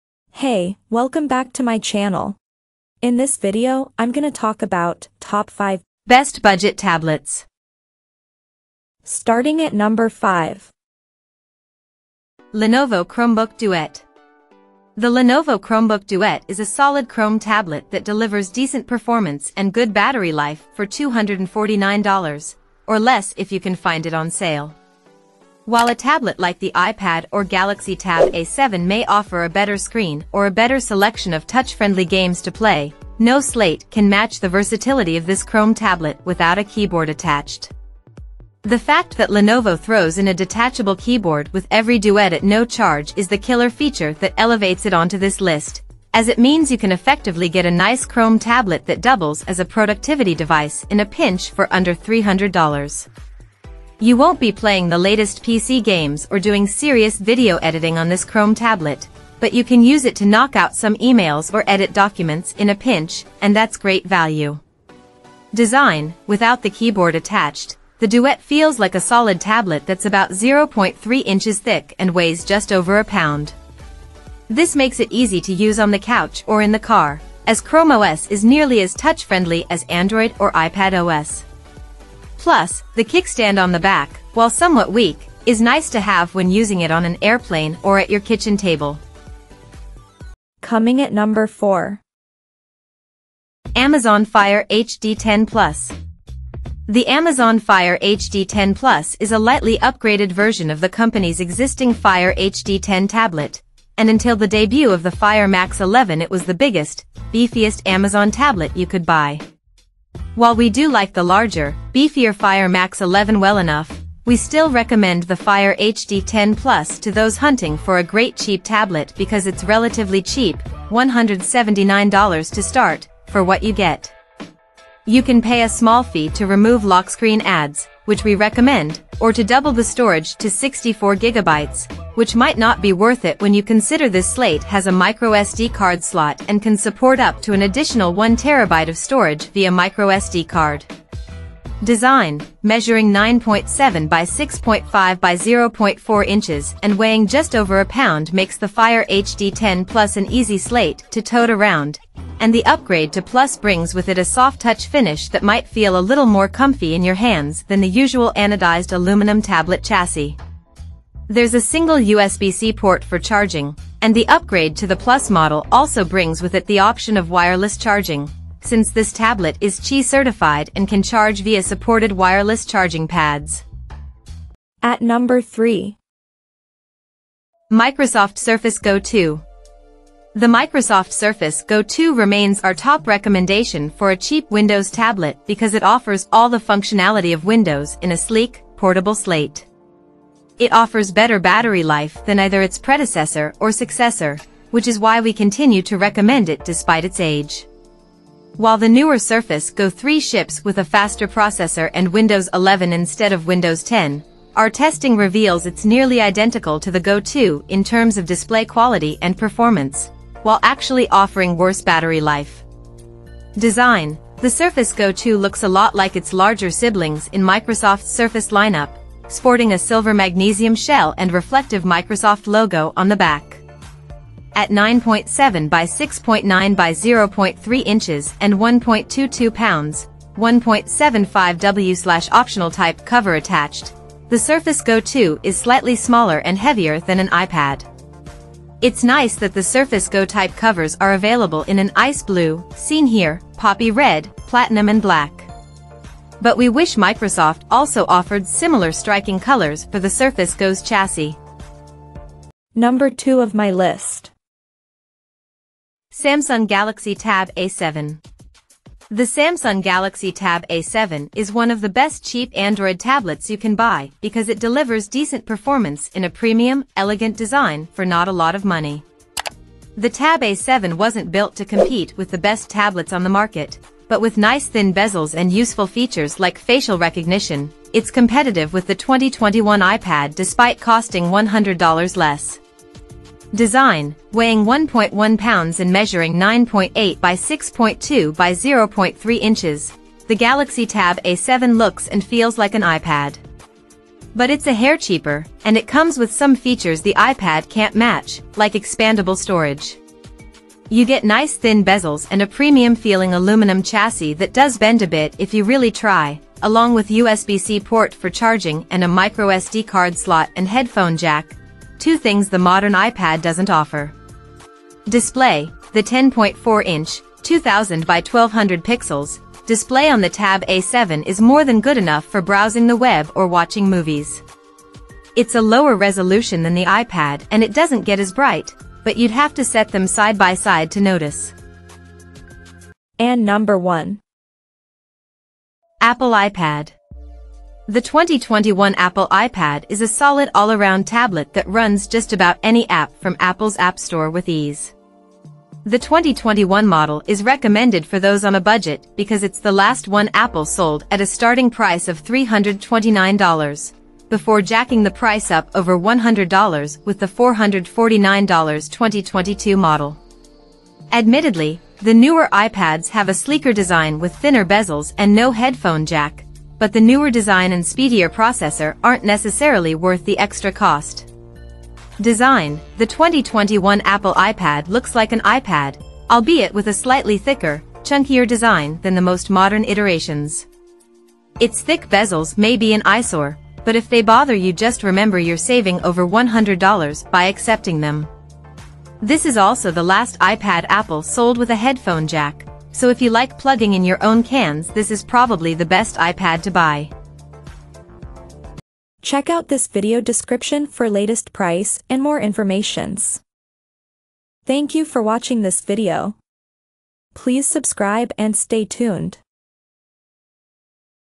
5 Best Budget Tablets in 2024
Here is the Updated Prices of the Best Budget Tablets in 2023!

Things that we mentioned in this video:
1. Apple iPad  - B09G9FPHY6
2. Samsung Galaxy Tab A7 - B08GH2C64R
3. Microsoft Surface Go 2 - B086QRQVD5
4. Amazon Fire HD 10 Plus - B08F6FYN6B
5. Lenovo Chromebook Duet - B0856QVM2F